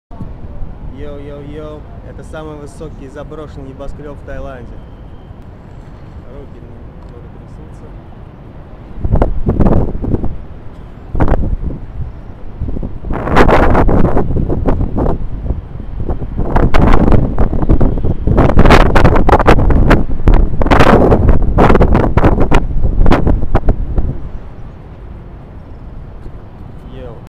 Заброс Бангкок
Бангкок Sathorn Unique, самый высокий заброшенный небоскреб-призрак в мире.